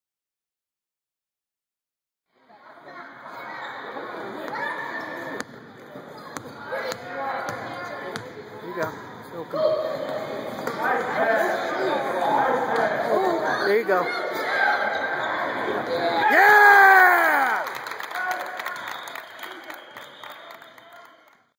2 pointer
PJ scores in his first basketball game of the season!